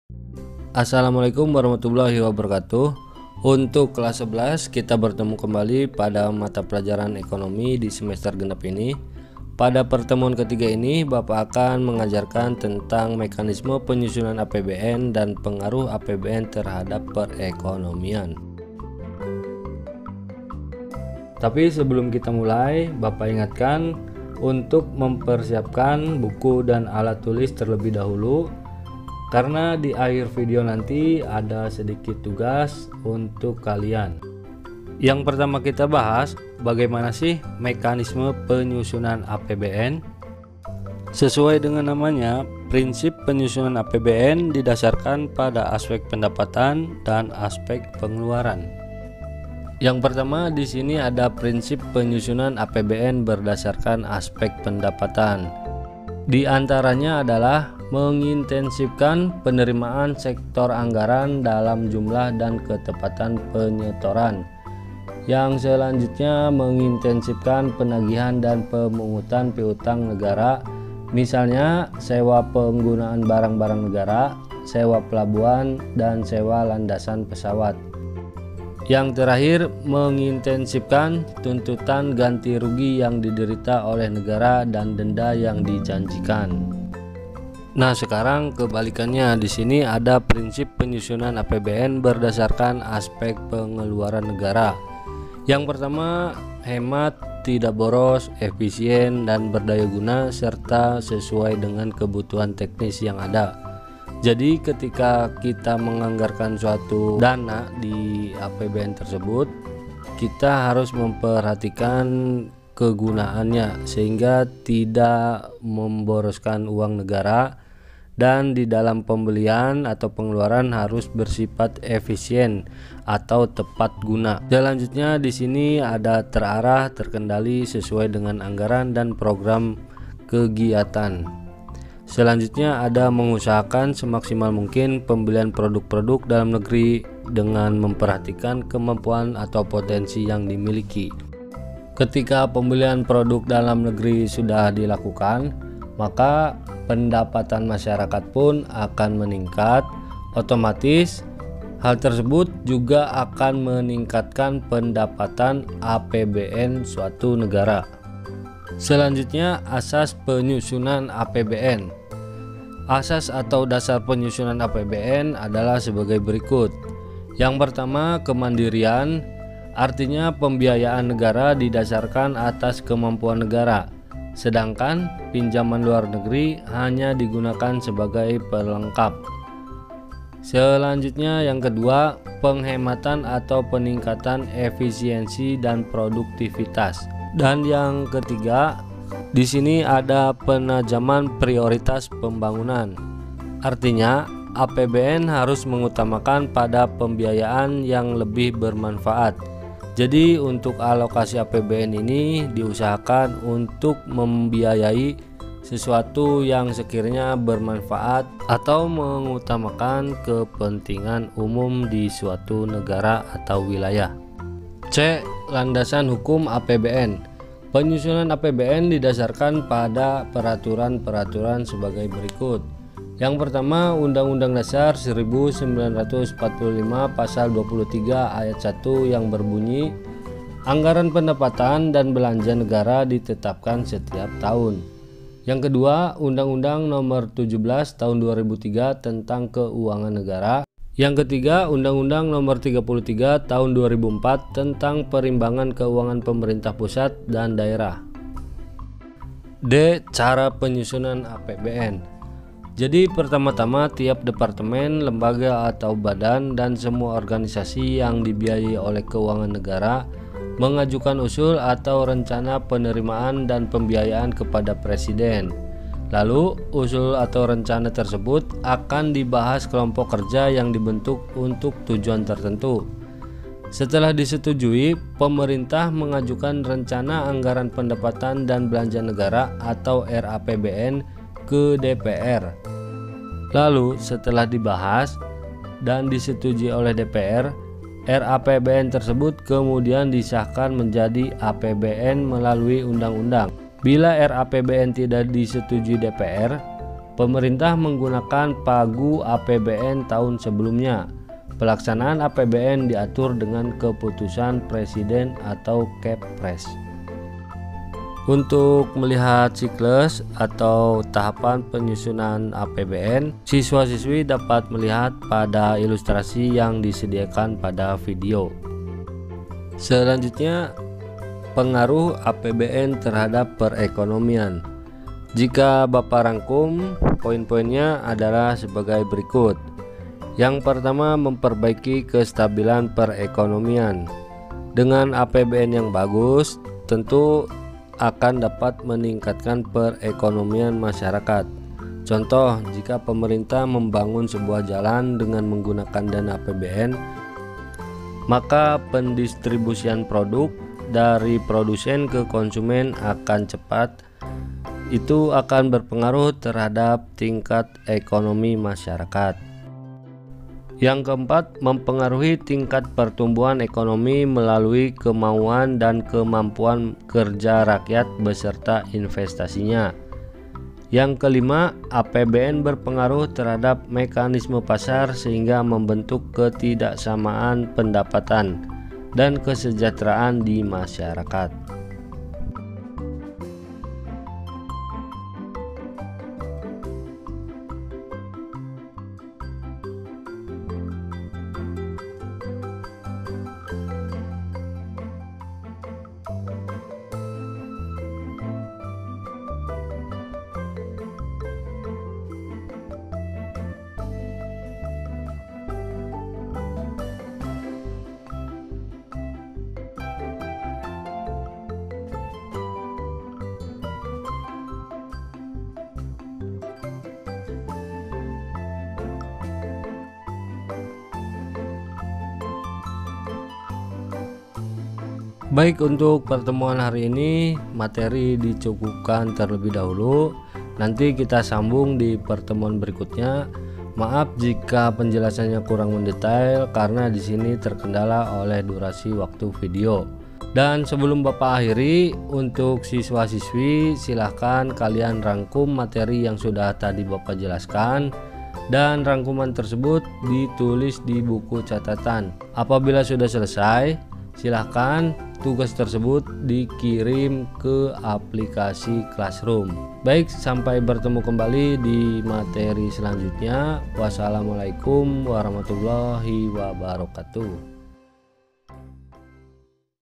Mekanisme Penyusunan APBN dan Dampak APBN untuk Perekonomian - Ekonomi kelas XI / Semester Genap
Pelajaran : Ekonomi
Kelas : XI / Genap
Materi : APBN dan APBD
Sub materi : Mekanisme penyusunan APBN, dan dampak APBN untuk Perekonomian

PERTEMUAN KE - 3